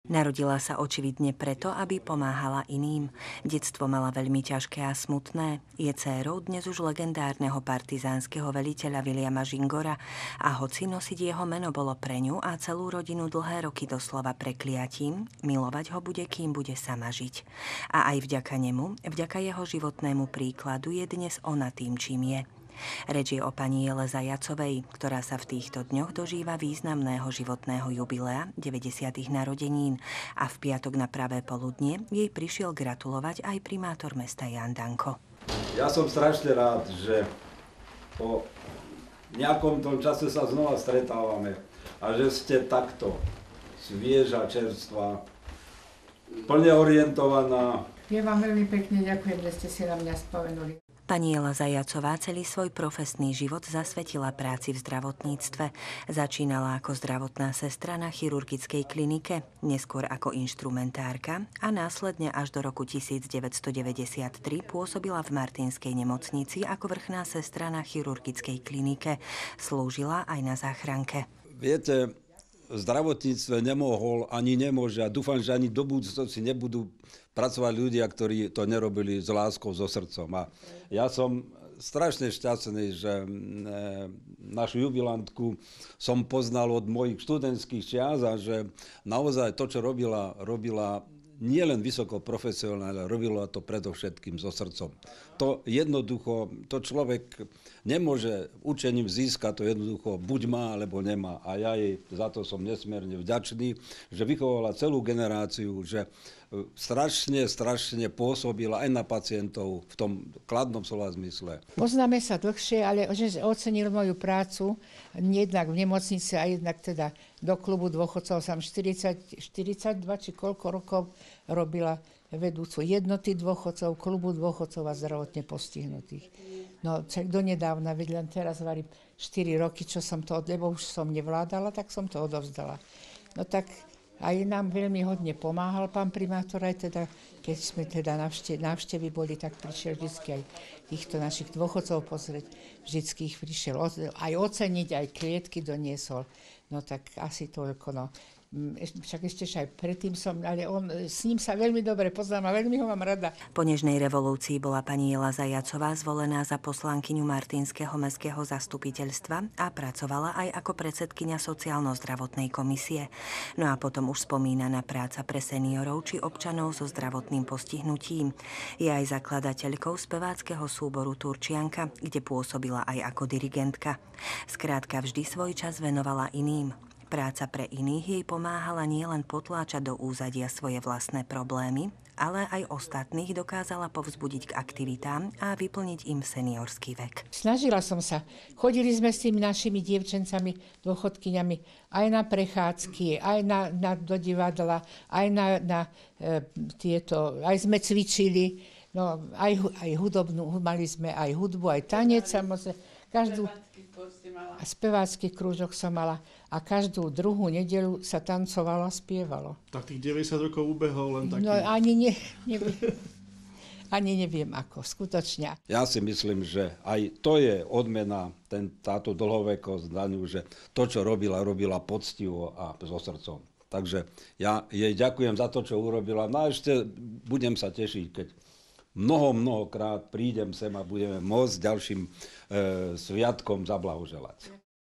Jela Zajacová, vzácna dáma sa dožíva významného životného jubilea
Celý život žije preto, aby pomáhala iným. Aj po vzore svojho otca boli jej vlastné záujmy vždy v úzadí a vždy sa zamerala na to, aby spokojní a šťastní boli tí okolo nej. V týchto dňoch sa dožíva významného životného jubilea a gratulovať jej prišiel aj primátor mesta Ján Danko, ktorý ju tiež vníma ako výnimočnú dámu. Pani Jela celý svoj profesný život zasvätila práci v zdravotníctve, pracovala ako zdravotná a neskôr ako vrchná sestra v martinskej nemocnici. Po odchode na dôchodok sa výrazne angažovala v práci v prospech seniorov a občanov so zdravotným postihnutím.